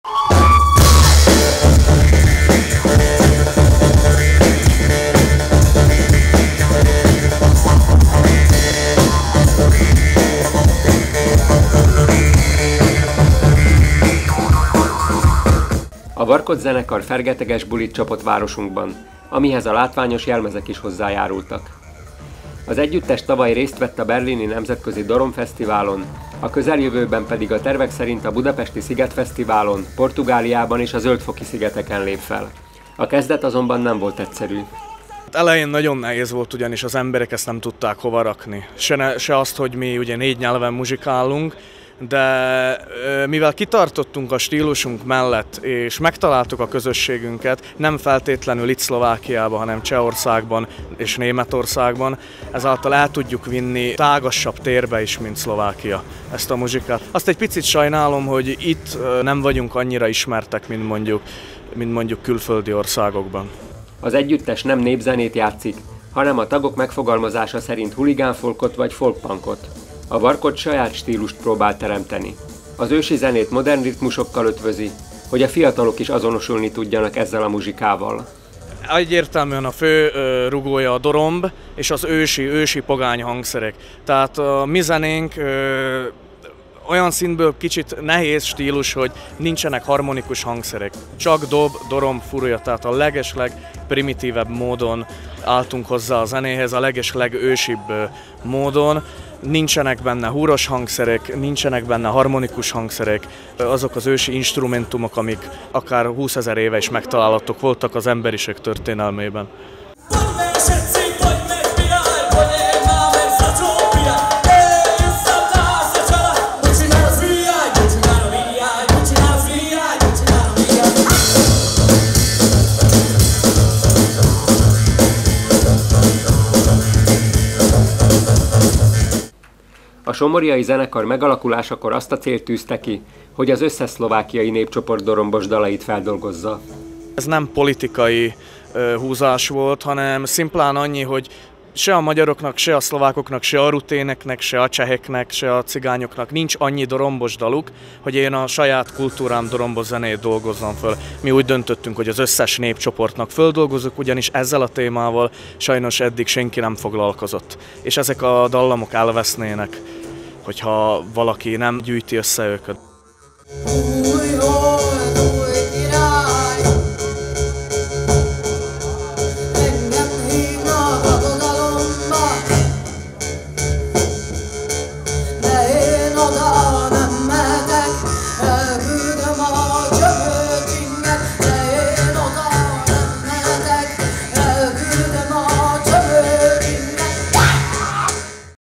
Skupina Varkocs oslavovala jubileum úspešným koncertom
Skupina Varkocs oslávila piate výročie svojho vzniku úspešným dunajskostredským koncertom. Šamorínska kapela v sobotu večer vystúpila v klube NFG pred početným obecenstvom. Trojčlennú formáciu v tento večer doplnili dvaja hostia. Ťažiskom hudby skupiny je drumbľa, prastarý hudobný nástroj, na ktorom sa hrá brnkaním a pískaním.